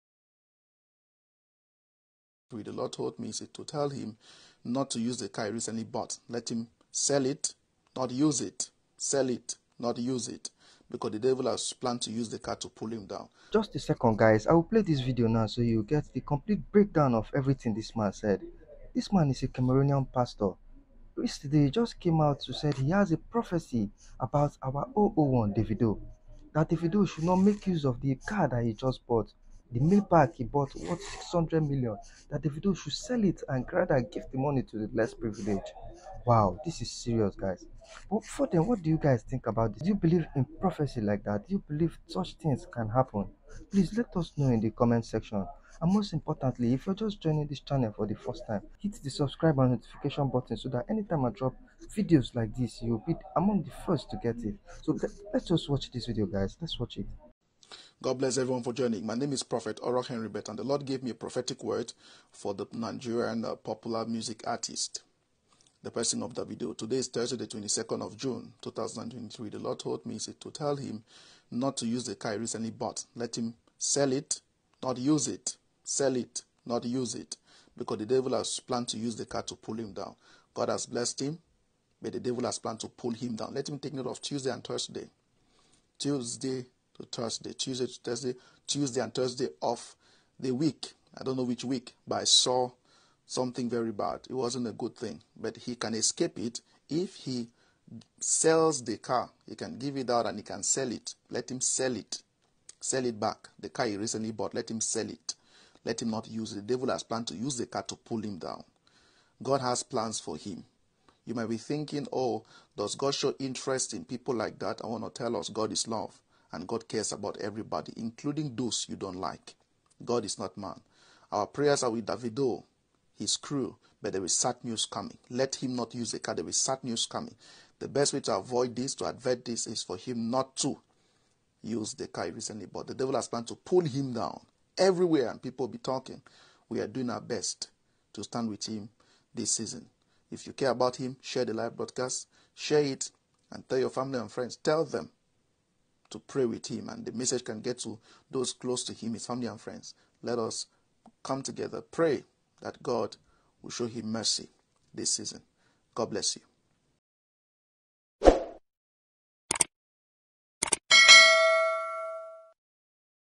Prophet Shares HORRIFIC Prophecy About DAVIDO and his new Maybach
A prophet has urged Davido to get rid of his newly acquired Maybach he flew into the country recently as it could be his doom.  The cleric revealed that the Lord said the devil wants to use the car to pull him down.